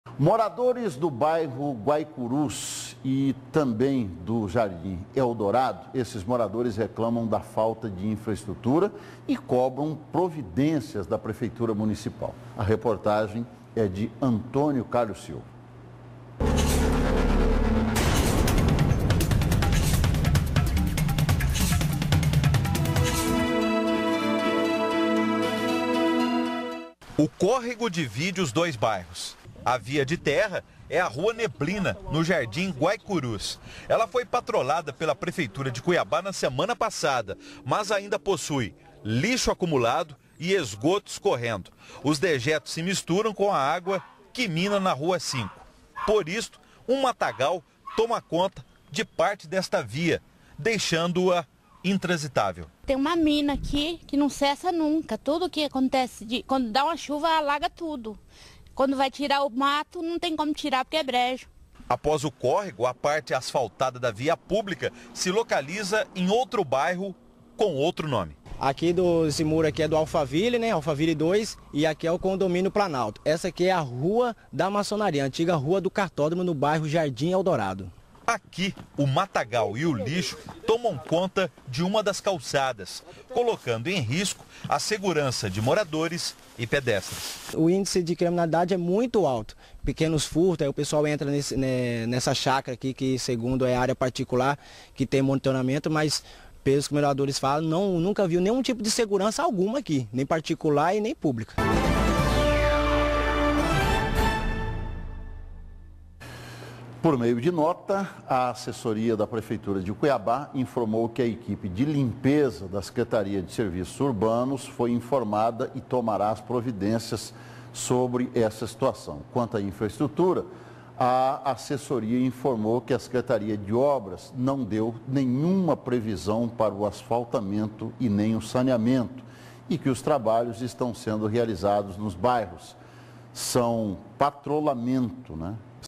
População cobra infraestrutura
Moradores do bairro Guaicurus e Jardim Eldorado exigem providências do poder público municipal.